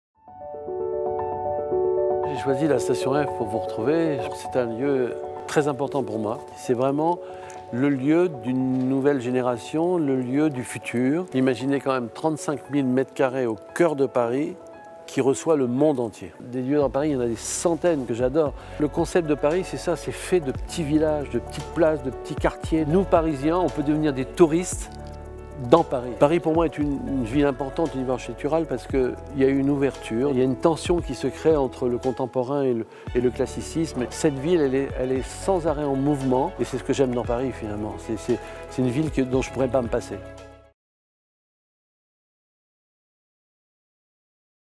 PARISCREATION with Jean-Michel Wilmotte
"It's a city I could not do without."

We met French architect, Jean-Michel Wilmotte, at Station F and he shared his love for Paris and Parisian architecture with us.

Love our ‘Paris je t’aime’ videos?